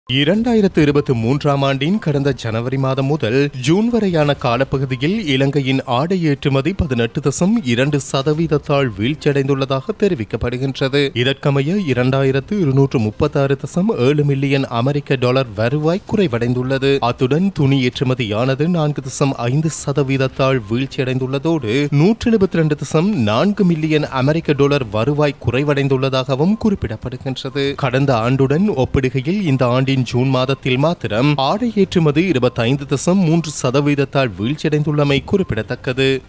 ஆடை ஏற்றுமதியில் வீழ்ச்சி
2023ம் ஆண்டின் கடந்த ஜனவரி முதல் ஜூன் வரையான காலப்பகுதியில், இலங்கையின் ஆடை ஏற்றுமதி, 18.2 சதவீதத்தால் வீழ்ச்சியடைந்துள்ளதாக தெரிவிக்கப்படுகின்றது.

இதற்கமைய, 2 ஆயிரத்து 236.7 மில்லியன் அமெரிக்க டொலர் வருவாய் குறைவடைந்துள்ளது.

அத்துடன், துணி ஏற்றுமதியானது 4.5 சதவீதத்தால் வீழ்ச்சியடைந்துள்ளதோடு, 172.4 மில்லியன் அமெரிக்க டொலர் வருவாய் குறைவடைந்துள்ளதாகவும் குறிப்பிடப்படுகின்றது.

கடந்த ஆண்டுடன் ஒப்பிடுகையில், இந்த ஆண்டின் ஜூன் மாதத்தில் மாத்திரம், ஆடை ஏற்றுமதி 25.3 சதவீதத்தால் வீழ்ச்சியடைந்துள்ளமை குறிப்பிடத்தக்கது.